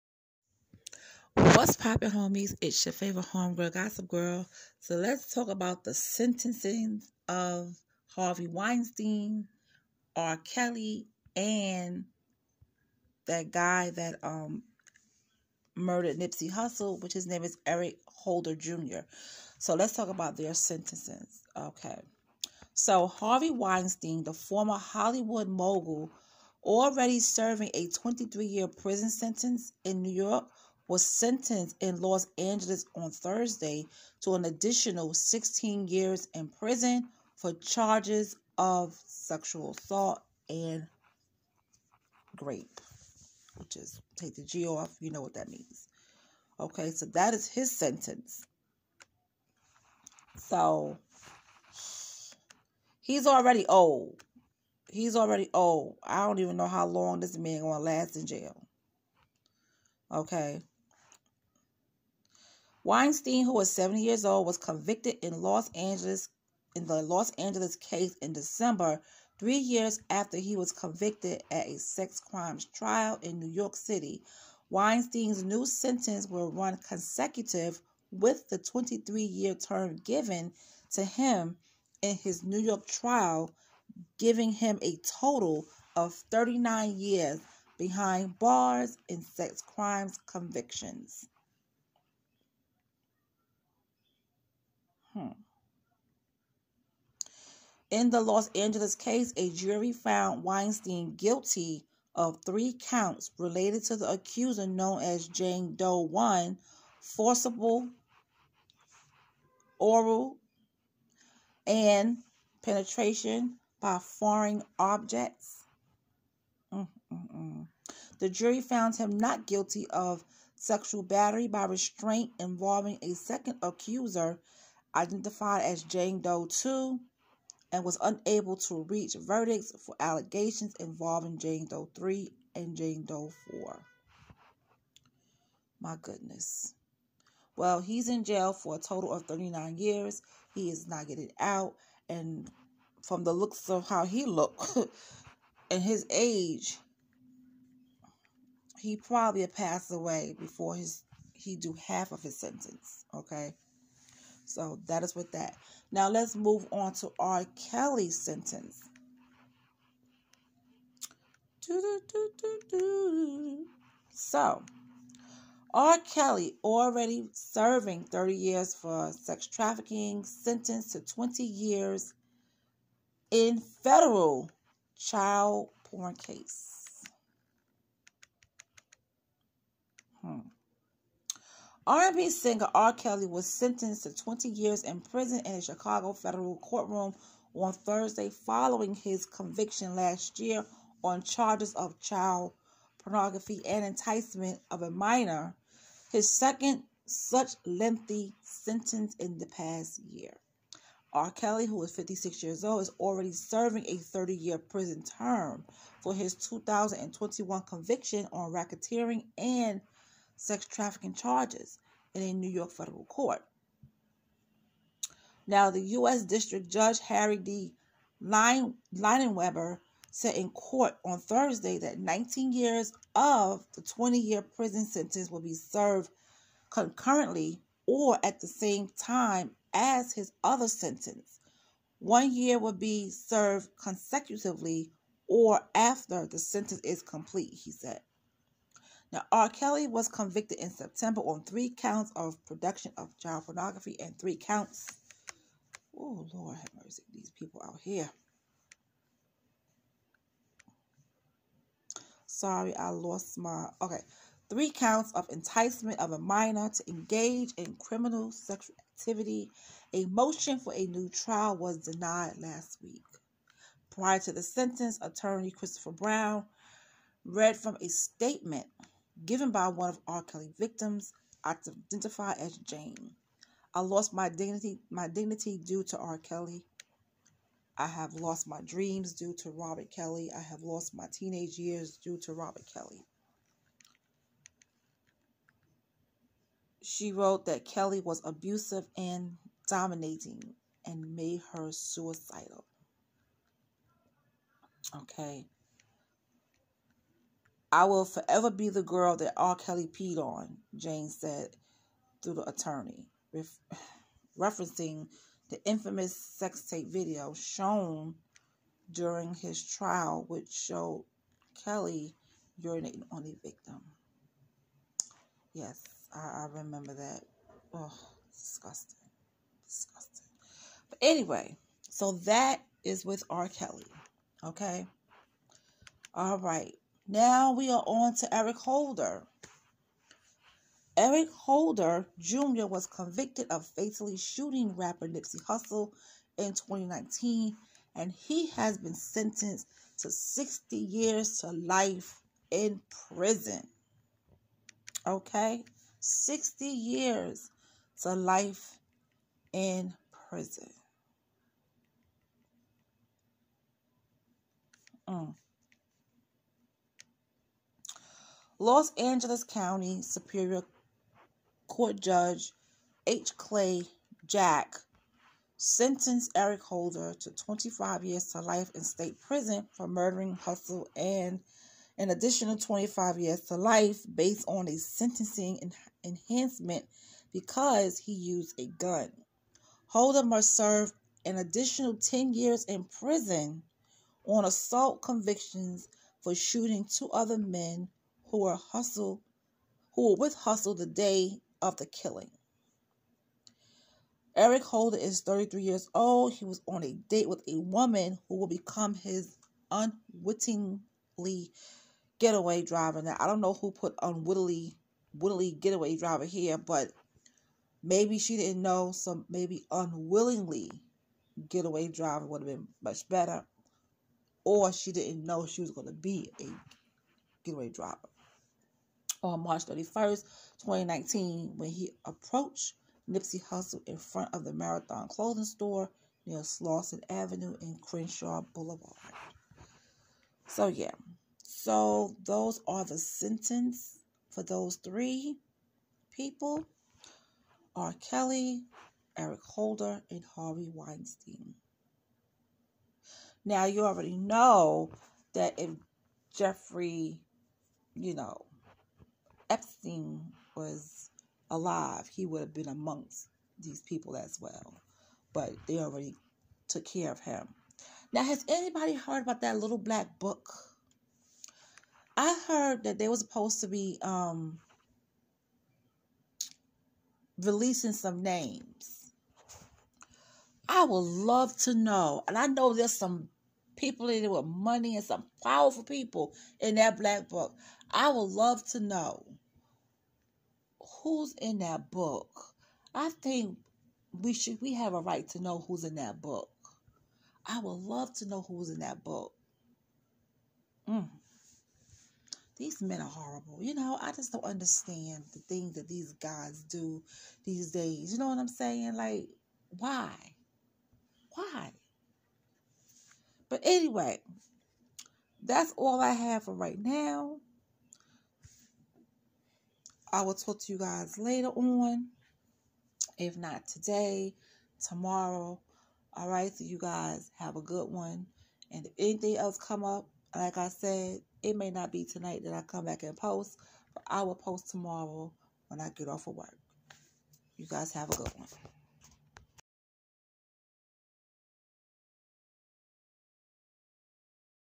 Harvey Weinstein | R Kelly | Eric Holder Jr | SENTENCED
Sentencing has happened for Harvey Weinstein, R Kelly, Eric Holder Jr.